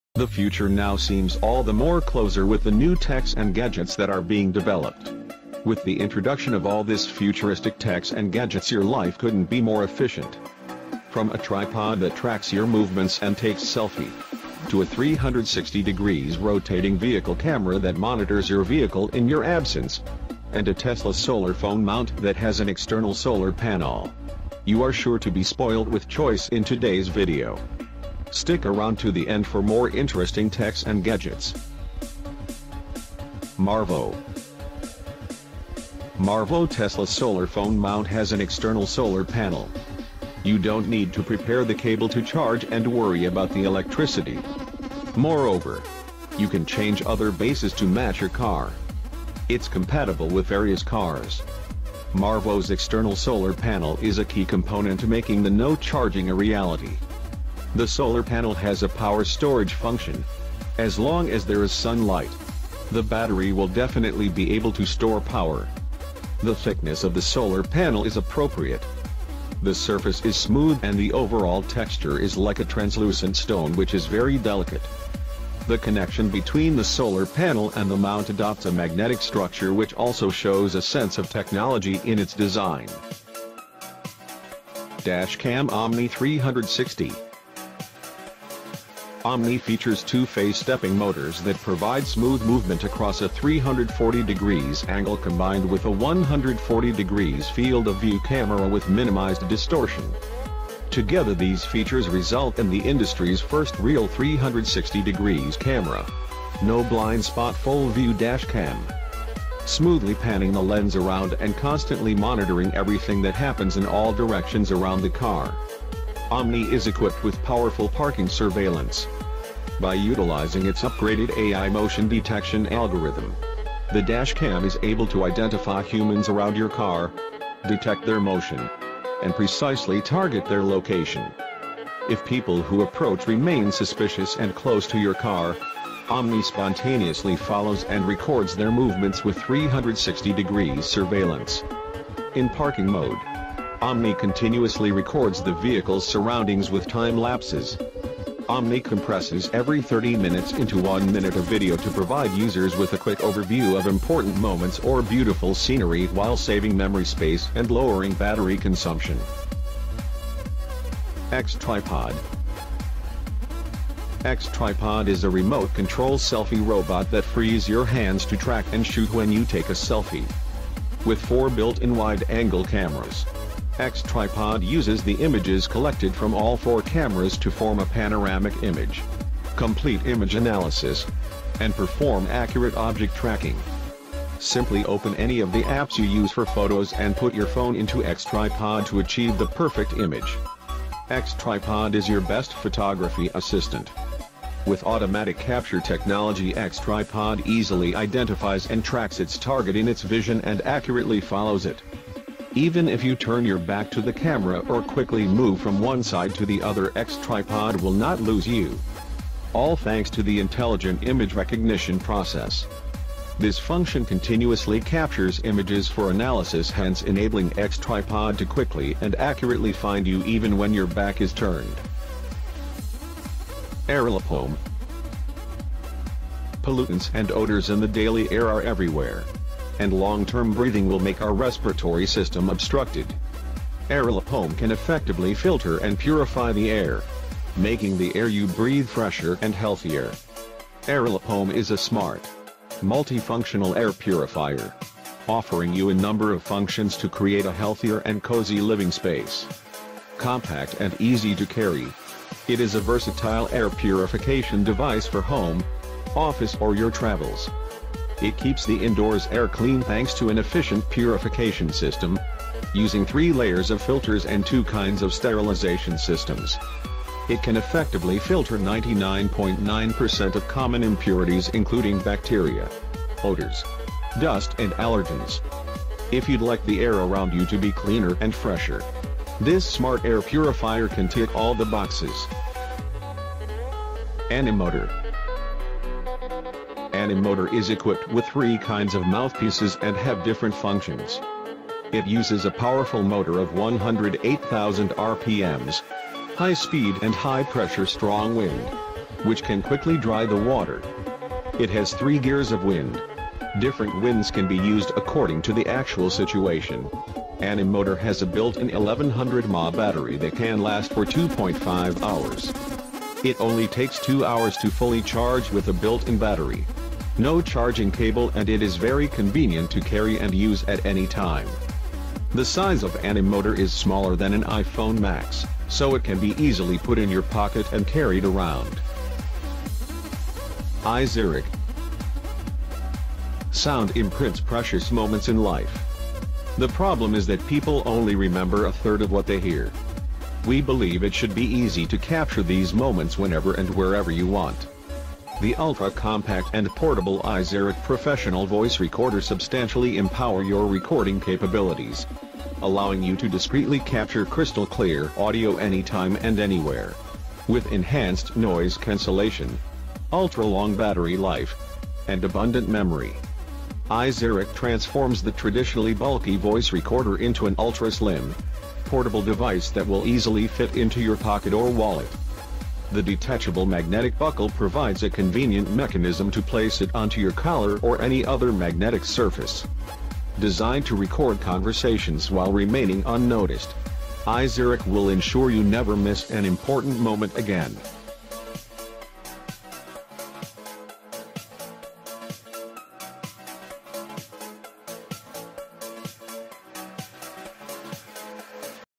Top 6 High Tech Gadgets And Inventions In 2022 You Must Have In Your Home
The future now seems all the more closer with the new techs and gadgets that are being developed, with the introduction of all this futuristic techs and gadgets your life couldn't be more efficient. From a tripod that tracks your movements and takes selfie, to a 360° Rotating Vehicle camera that monitors your vehicle in your absence, and a Tesla Solar Phone Mount that has an external solar panel, you are sure to be spoilt with choice in today's video. stick around to the end for more interesting Techs and gadgets.